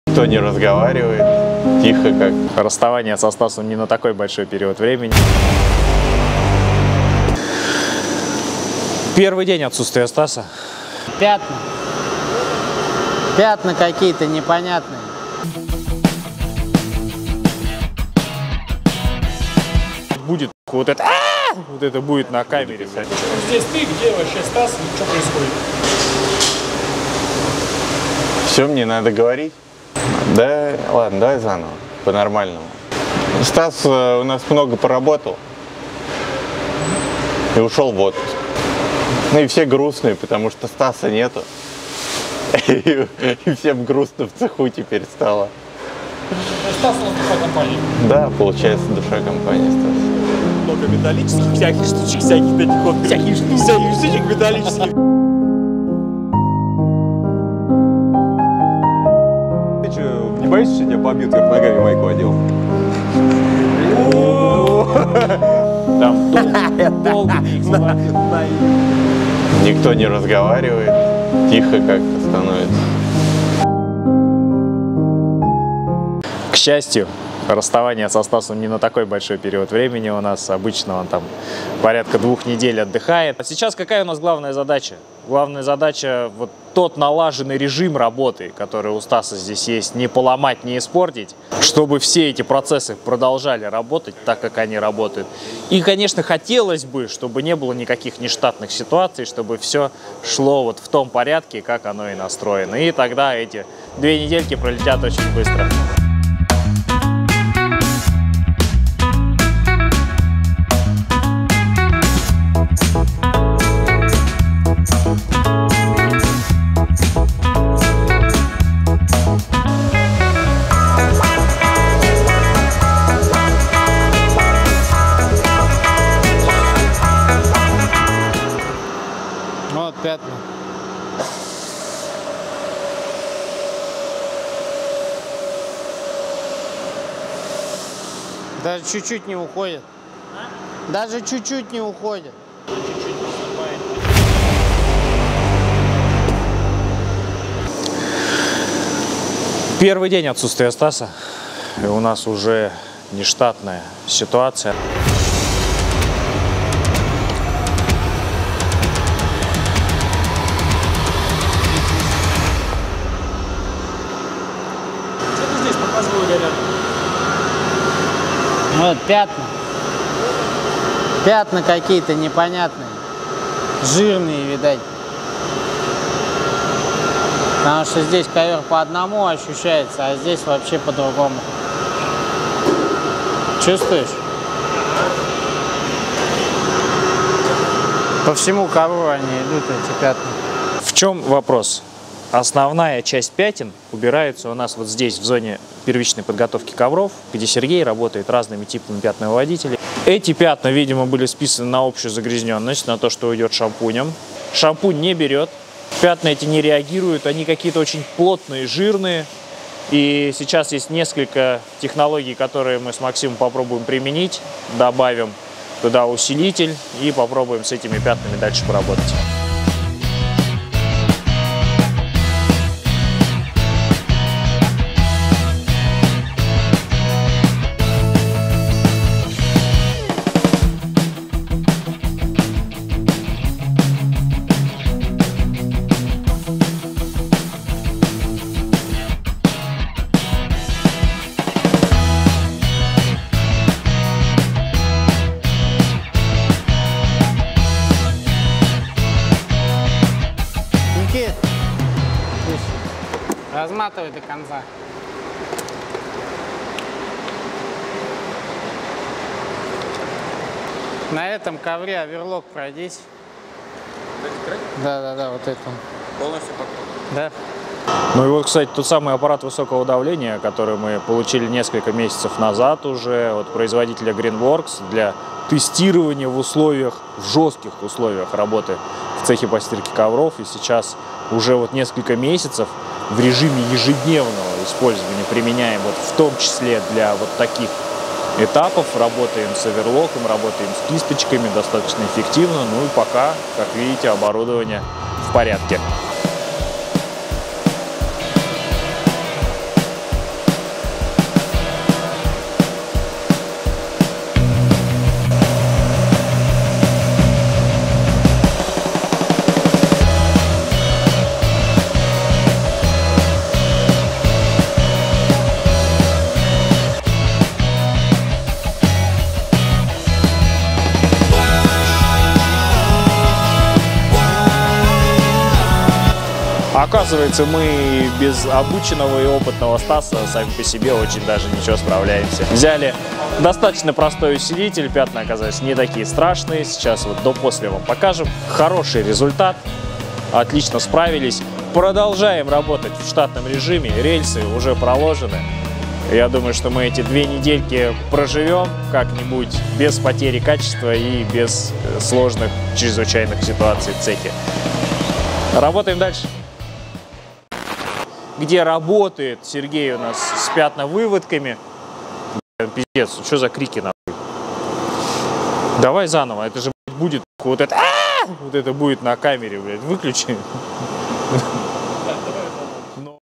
Жирные пятна на светлом ковре. Страх, сомнения и крики чаек / Стирка ковров Кемерово
Чуть не опозорились в первый же день отпуска начальника нашего цеха Станислава: на одном из ковров по всей площади были раскиданы какие-то непонятные пятна.

Справимся ли мы без Стаса?.. Результат в конце видео!

Смотрите с удовольствием!